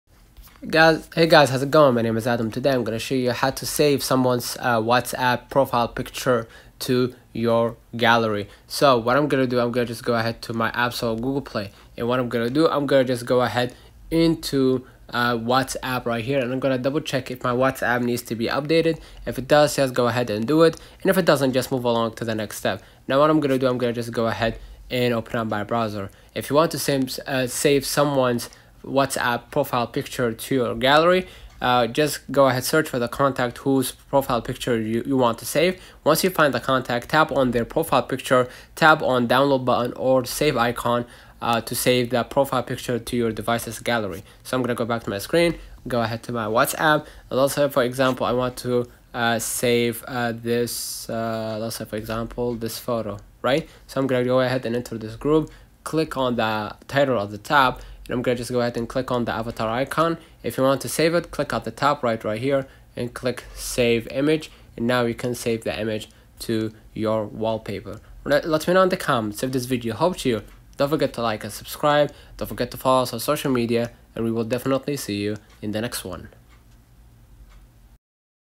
How to save someone's whatsapp profile picture to your gallery (2024)
How to save someone's whatsapp profile picture to your gallery

In this video i´ll show you How to save someone's whatsapp profile picture to your gallery. The method is very simple and clearly described in the video. follow all the steps & save someone's whatsapp profile picture to your gallery